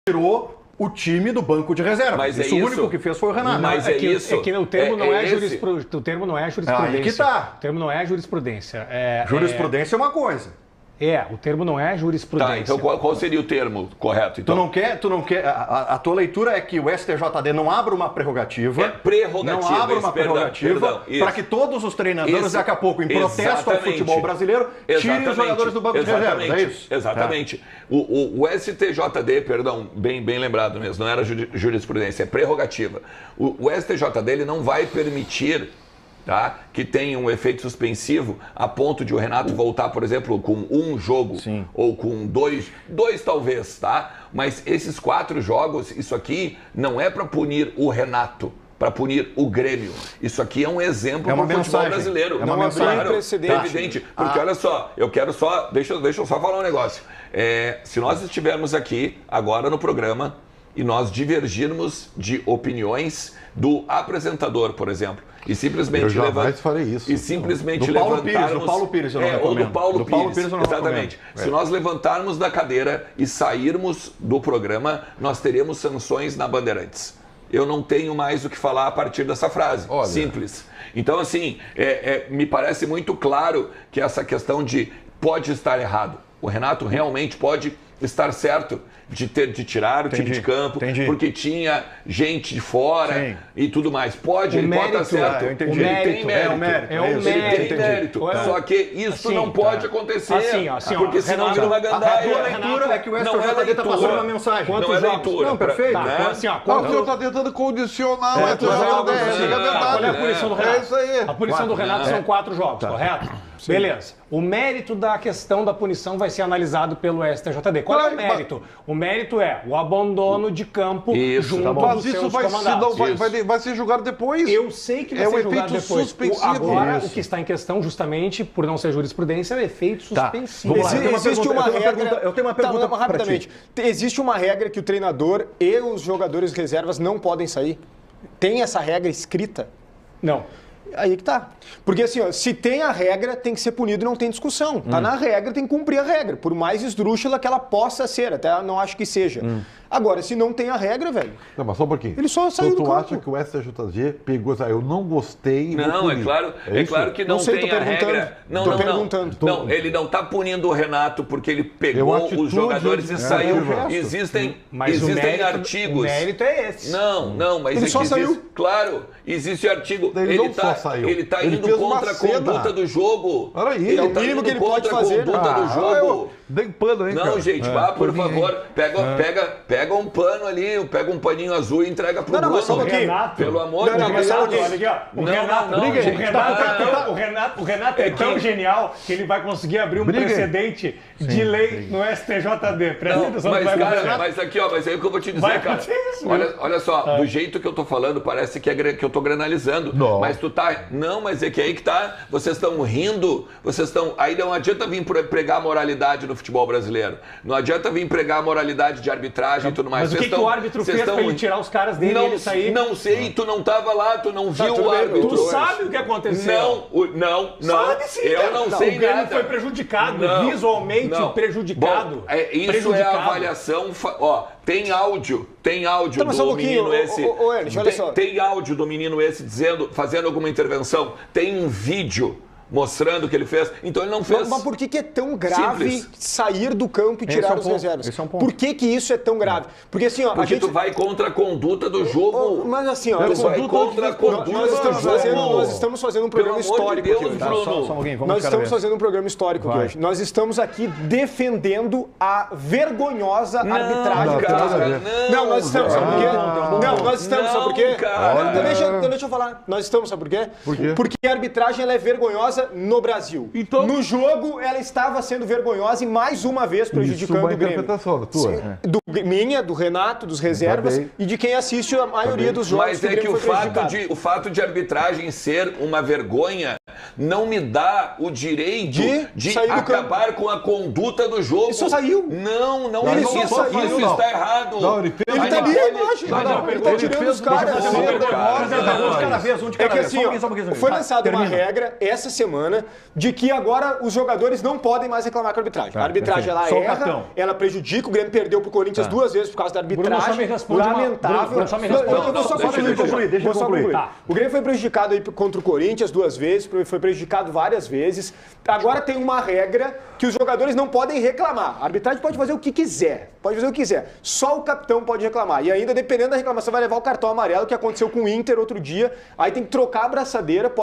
O GRÊMIO É PERSEGUIDO PELA ARBITRAGEM? RENATO MERECEU A PUNIÇÃO?
Opinião, análise e muita informação da dupla grenal!
Filipe Gamba, Ribeiro Neto, Alexandre Ernst, Pedro Espinosa e Bruno Soares com as últimas notícias de Grêmio e Inter!